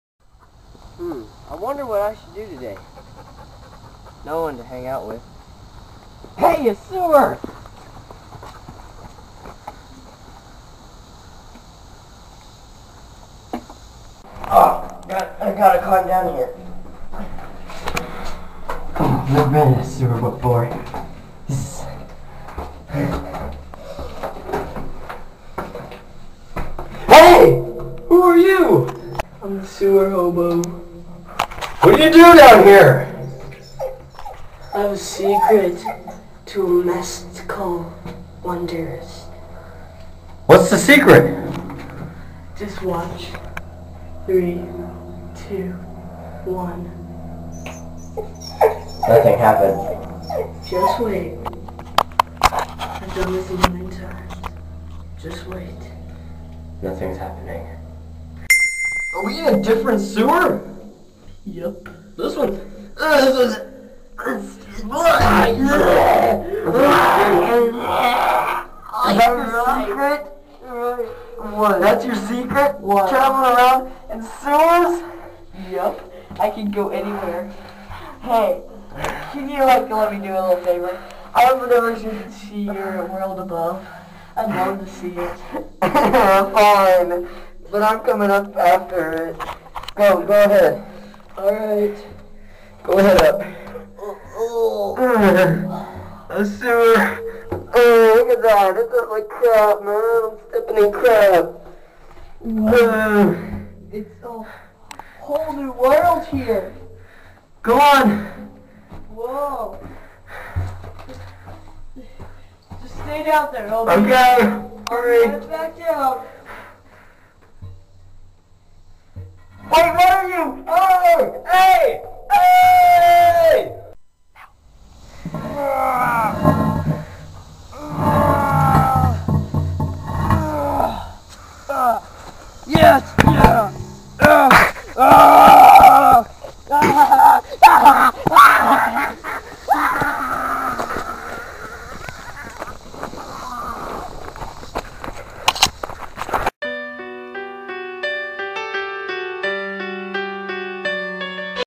The Sewer Hobo
A hobo that lives in the sewers!